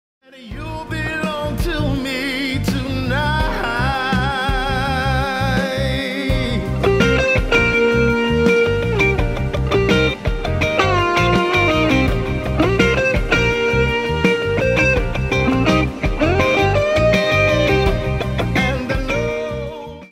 Strato Guitar Solo
Recording Guitar Solo for Dave Fitz production
Thanks to D'Orazio Strings,  Emme Guitars and Plectro for support